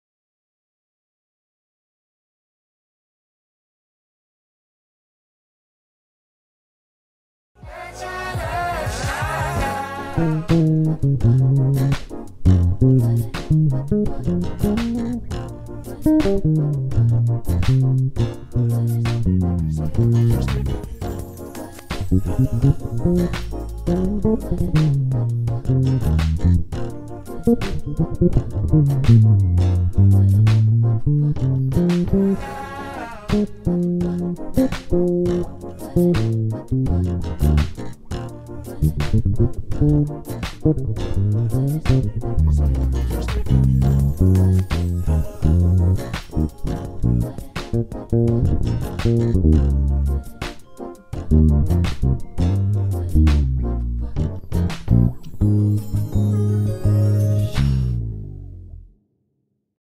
Jacob Collier Challenge - João Monteiro
Love Jacob. One of my favourite musicians. Having fun today exploring this vamp :) Using my N1 Bass from Marco Cortes